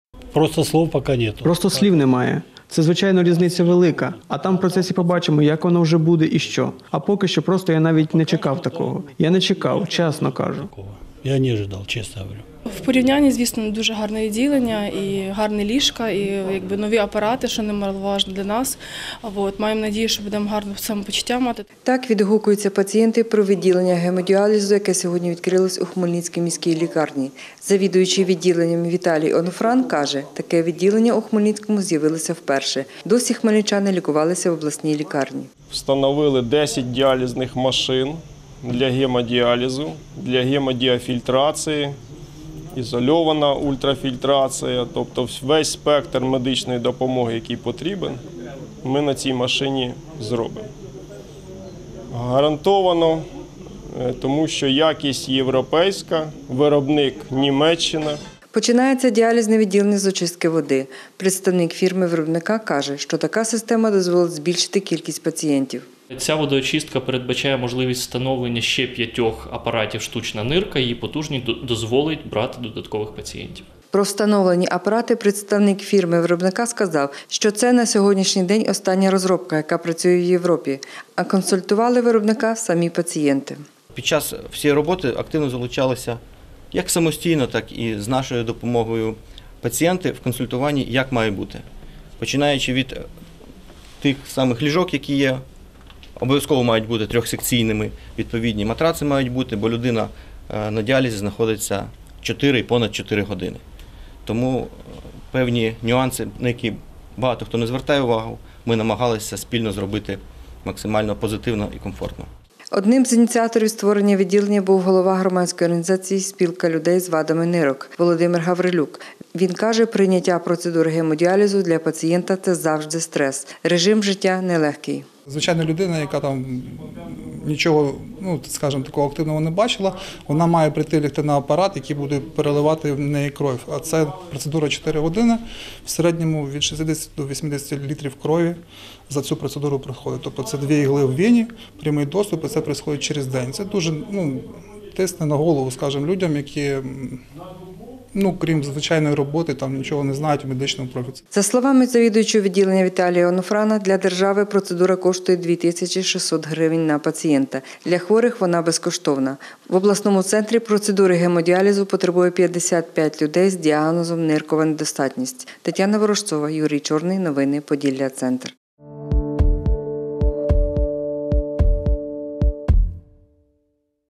Перше відділення гемодіалізу відкрили у Хмельницькій міській лікарні .
Воно розраховане на 55 пацієнтів. За словами  директора-компанії постачальника Ярослава Шатковського, у відділенні  встановлено сучасне європейське обладнання  вартістю понад 10 мільйонів гривень, це 10-ть апаратів для гемодіалізу і 10  багатосекційних  ліжок із спеціальними  матрацами, що підлягають вологій дезинфекції